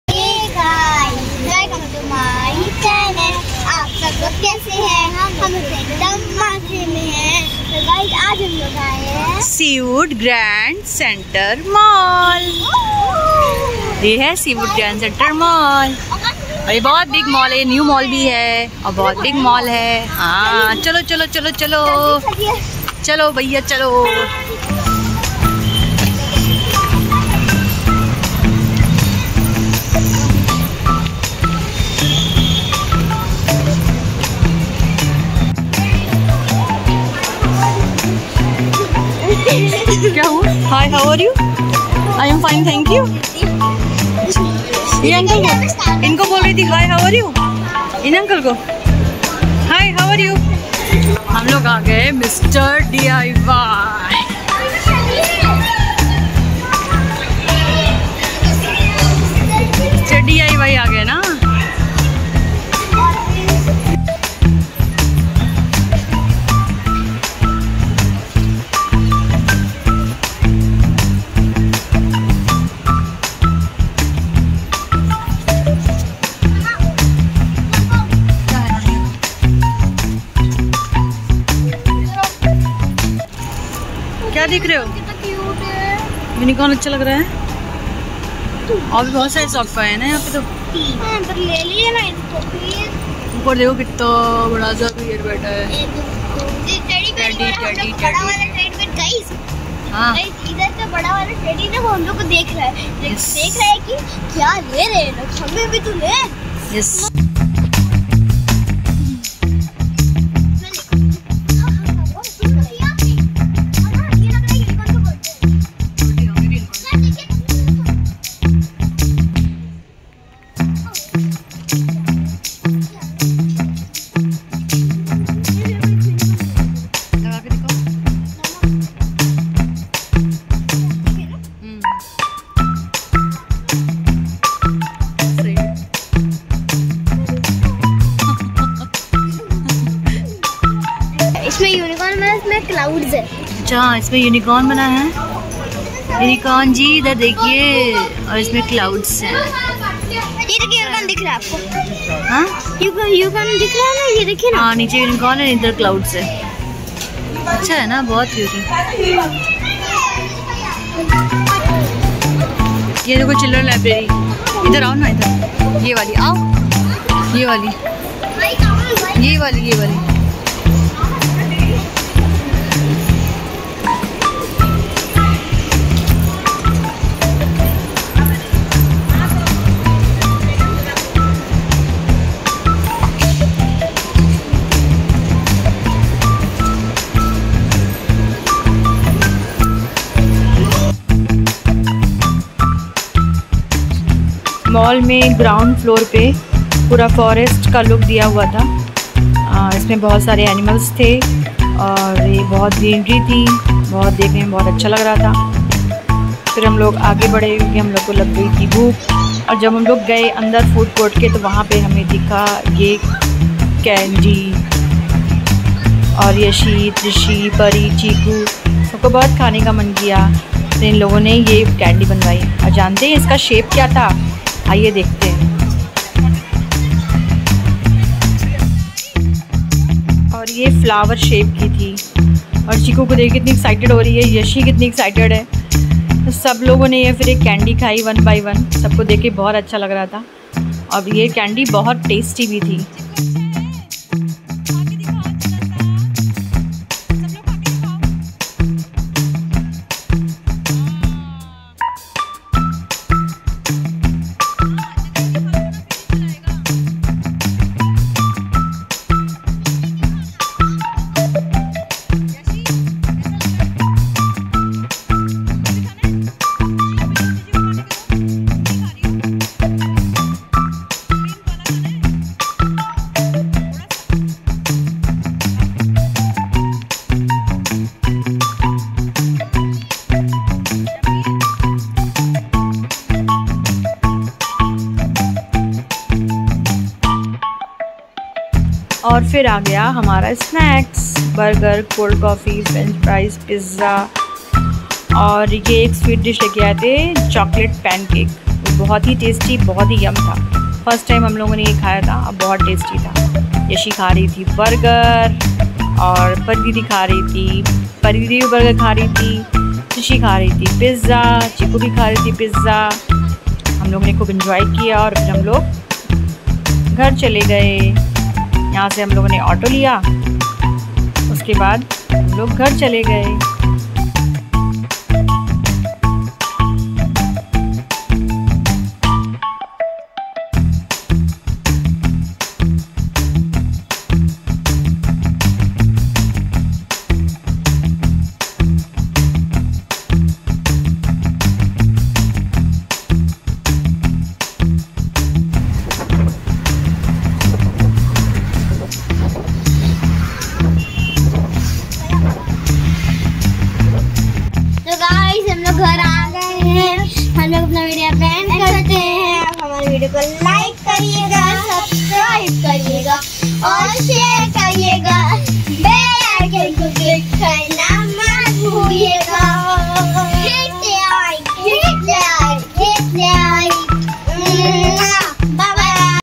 Mumbai Dayout Vlog 4 | Masti Time in Mr DIY | Flower Vali Cotton Candy 🍭 Khayi | Twinkle Sisters
Mumbai Dayout Vlog 4 | Mr DIY me ki shopping | Flower Vali Cotton Candy 🍭 Khayi | Twinkle Sisters